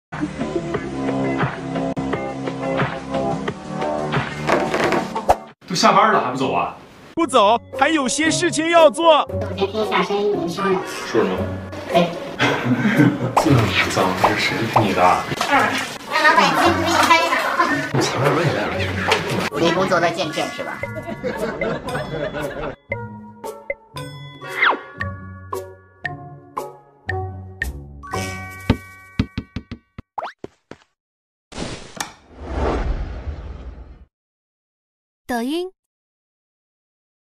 คลิปเบื้องหลังเลิกงาน ลุคนี้มาอีกแล้ว แพ้ทรงนี้จริงๆลุคนี้ตลอดไปเลยได้ป่าว! 🥰
[230907] XZ Studio Douyin Update

What stopped Sheng Yang from getting off work? 🤨